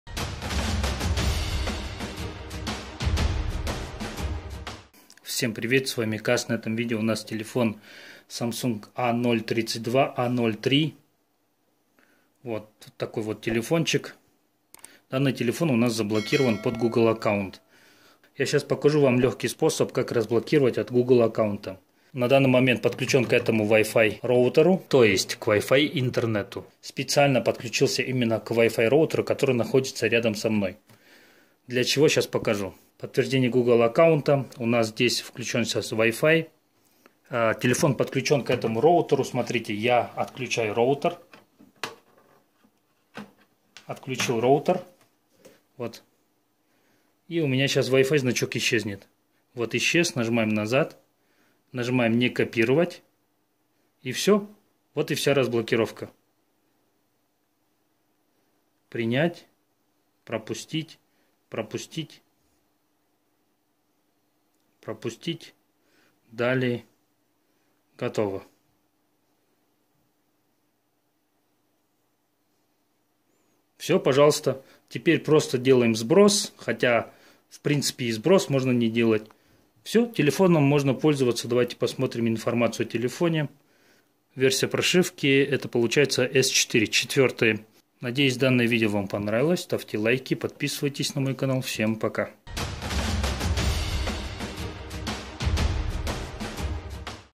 FRP! Samsung A032 U4 Новый метод! new method 2024. Google account bypass
На этом видео покажу как очень легко сделать разблокировку гугл аккаунта на телефоне Samsung.